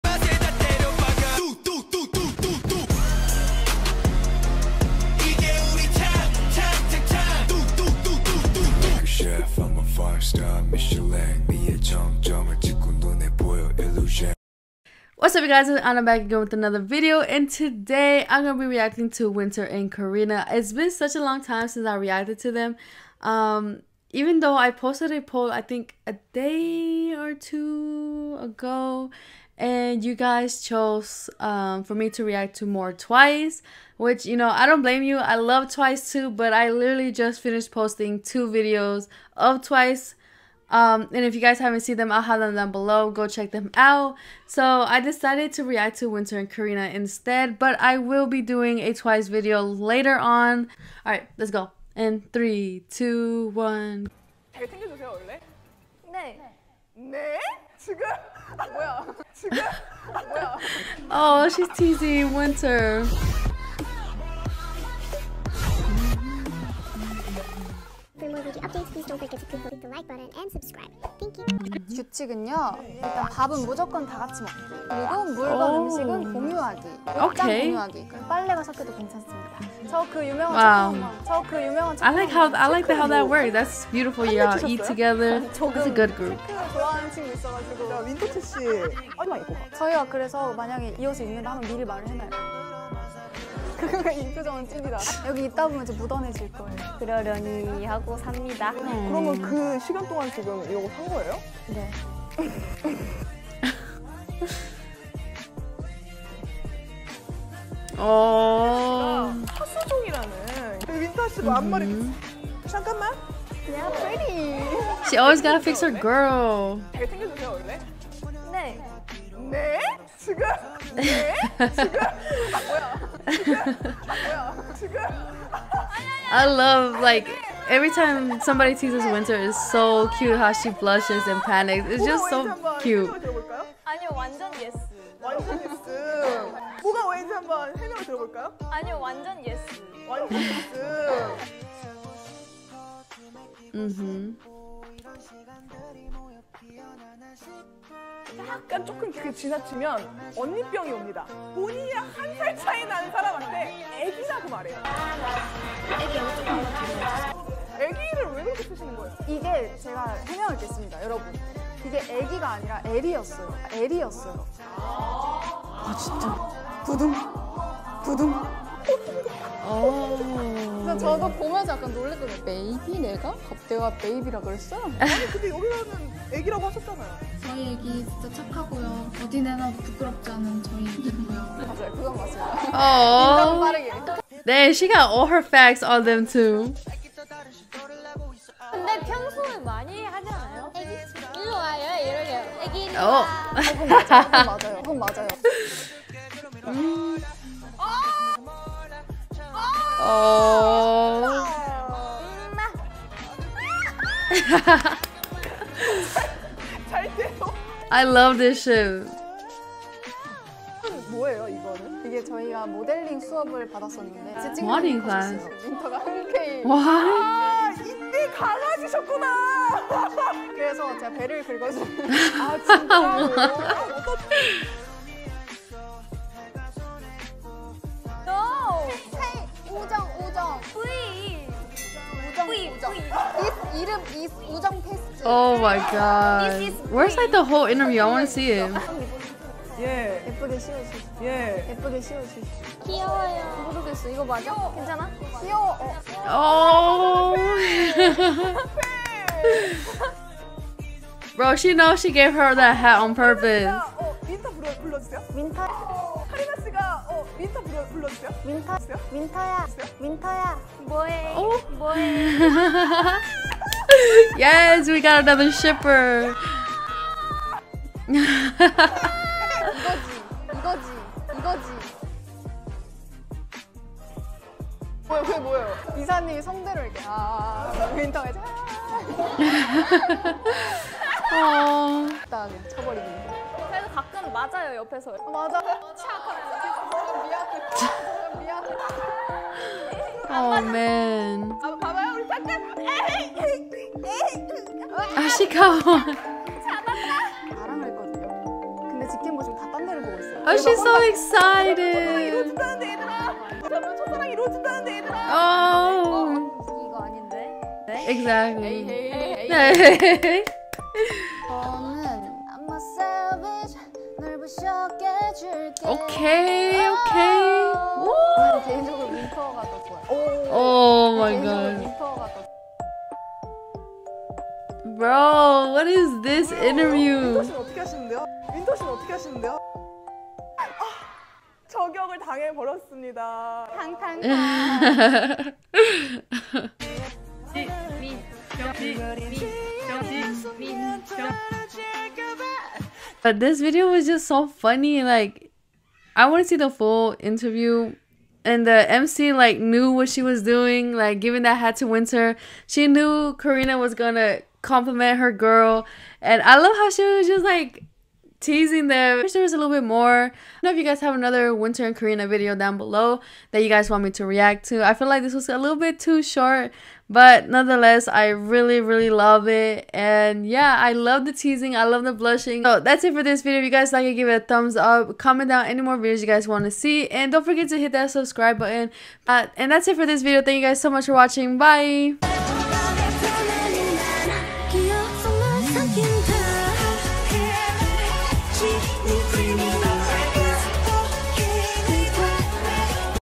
KARINA & WINTER JUST CONFESSED in an Interview! | REACTION
Welcome back to another video !! Happy Friday 💜
Sorry for a short video I had technical issues that I had to edit out .
But this video was SOO CUTE !! The Mc is definitely a SHIPPER !! I love how she making them blush and smile !! I will react to more video of them VERY SOON !!

MORE REACTIONS 💫

PS4-Thisis_Anna